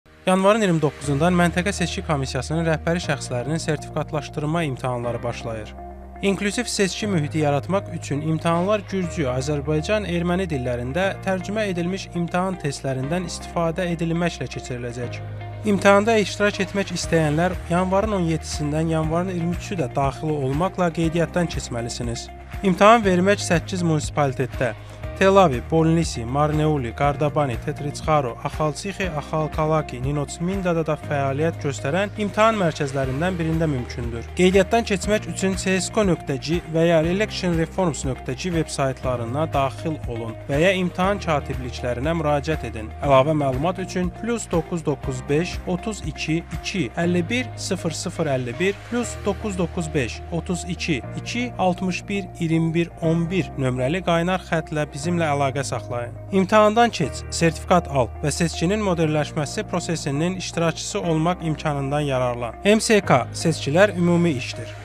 საუბნო საარჩევნო კომისიის ხელმძღვანელ პირთა სასერტიფიკაციო გამოცდები იწყება 2024 წელი
საუბნო საარჩევნო კომისიის ხელმძღვანელ პირთა სასერტიფიკაციო გამოცდები იწყება 2024 წელი (აზერბაიჯანულ ენაზე)